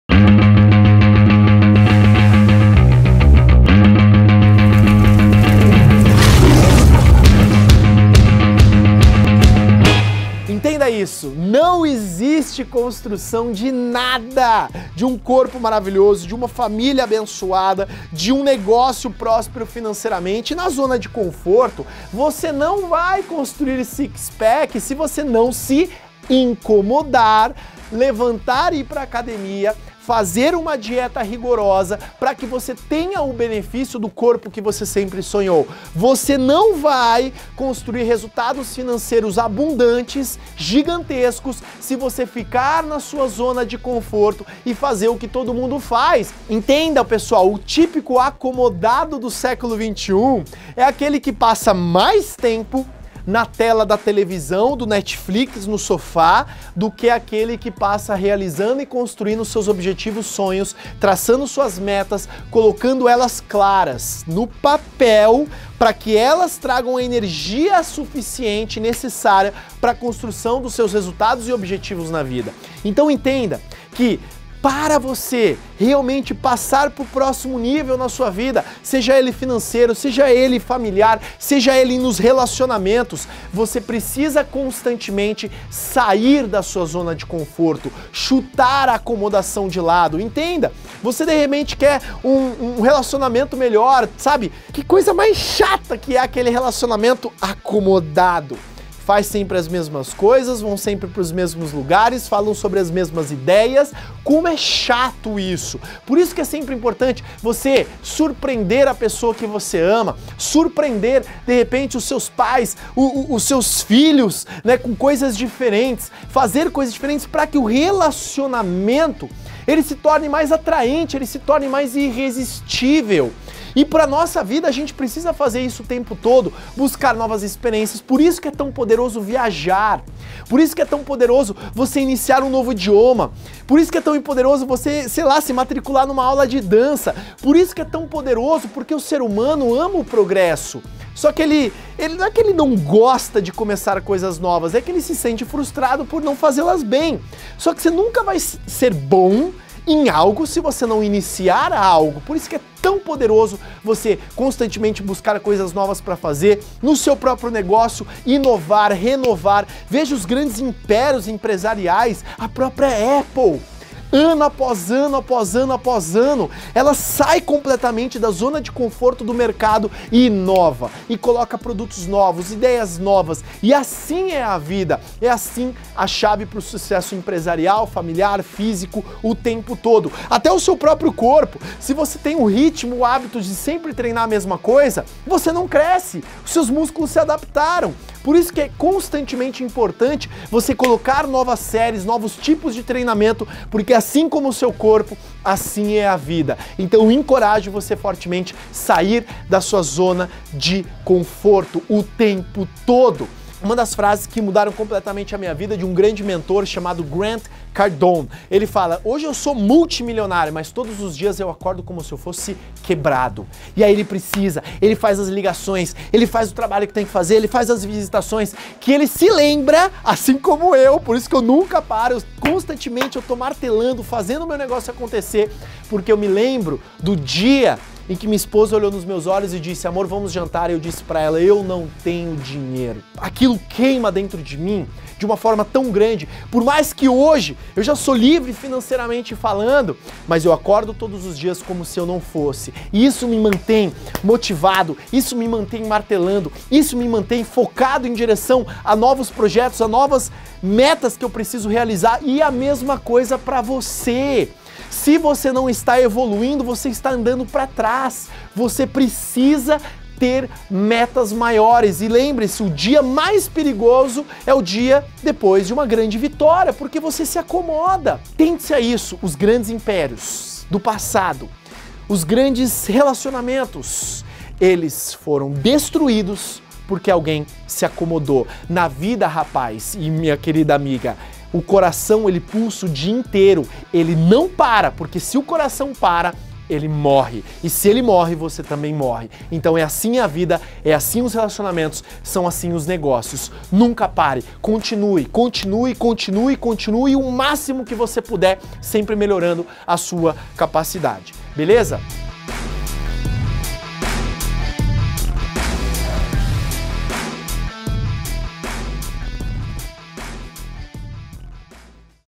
ZONA DE CONFORTO - Brancalion Mindset
🔴O SER HUMANO AMA O PROCESSO🔴

Você não vai construir algo poderoso se você não sair da zona de conforto, o típico acomodado do séculoXXI é aquele que faz o que a maioria faz ao invés de realizar seus objetivos, sonhos e traçar suas metas. Então entenda, para você passar para o próximo nível na sua vida, você PRECISA sair da zona de conforto.

A gente se vê no próximo vídeo ;D